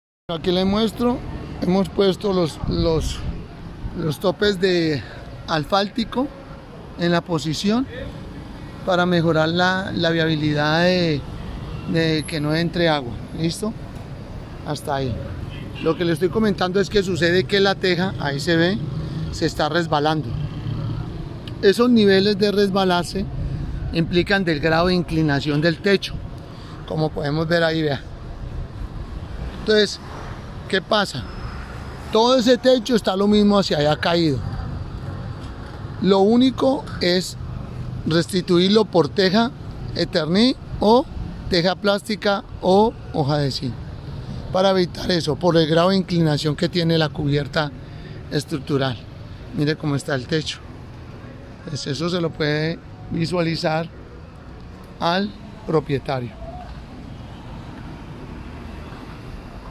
instalación sobre tejas y analiza su riego.
Cualquier tipo de instalación fotovoltaica o cualquier otra instalación que se requiera realizar en cubiertas, debemos ver la estructura, carga, capacidad y maniobrabilidad en cada una de ellas, así como su manejo para poder ser mucho más funcional. cualquier cambio o mejora en los techos.

Any type of photovoltaic installation or any other installation that is required to be carried out on roofs, we must see the structure, load, capacity and maneuverability in each of them, as well as their handling in order to be much more functional. any changes or improvements to the roofs.